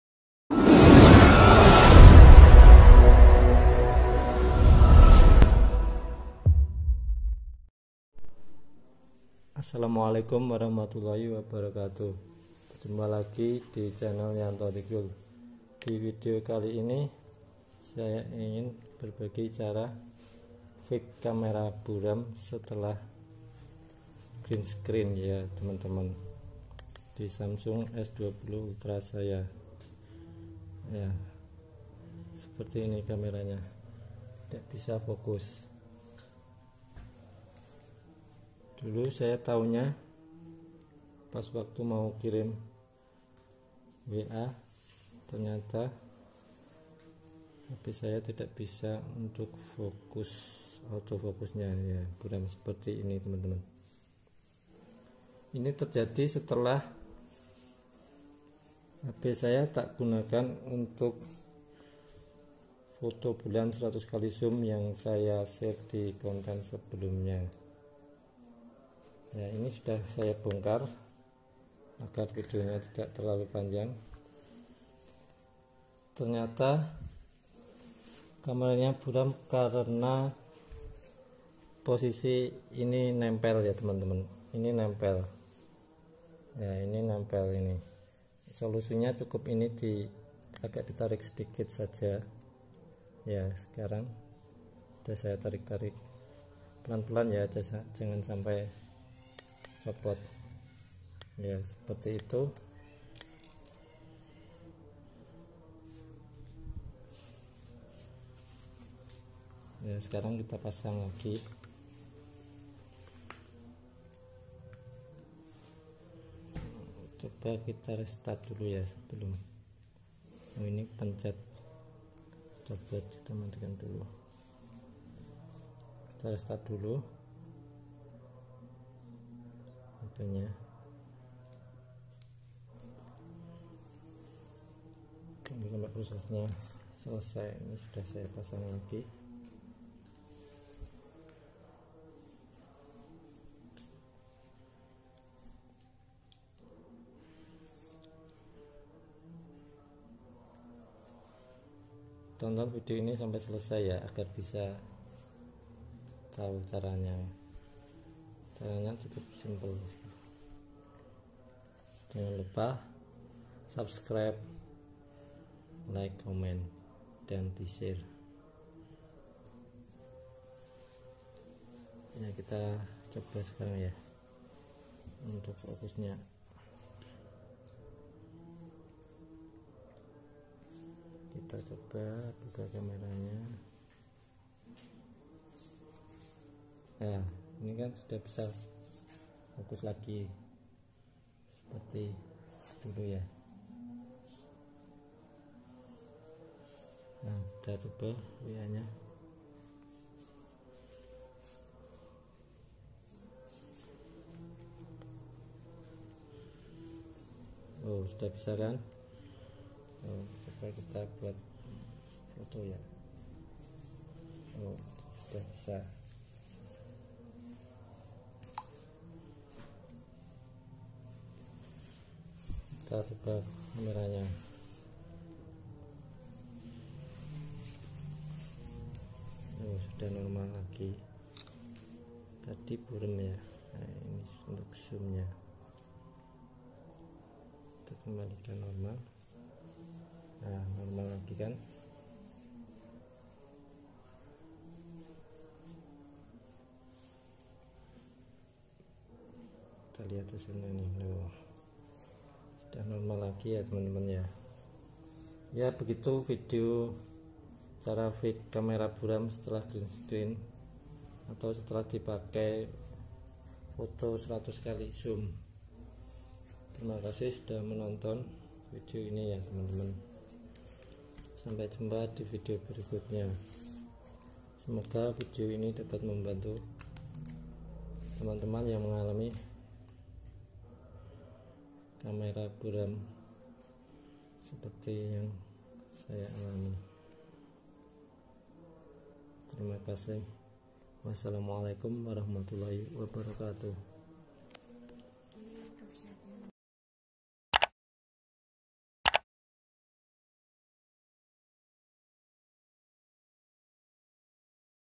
KAMERA BURAM & GAGAL FOKUS SETELAH GREEN SCREEN SAMSUNG GALAXY S20 ULTRA
FIX KAMERA BURAM & GAGAL FOKUS ‼️ SETELAH GREEN SCREEN SAMSUNG GALAXY S20 ULTRA

Divideo kali ini saya ingin berbagi pengalaman tentang fix kamera buram atau blur dan gagal fokus samsung s20 ultra setelah green screen atau setelah dipakai zoom 100x.

Bagi teman - teman yang pernah mengalaminya silahkan tonton dan pelajari caranya.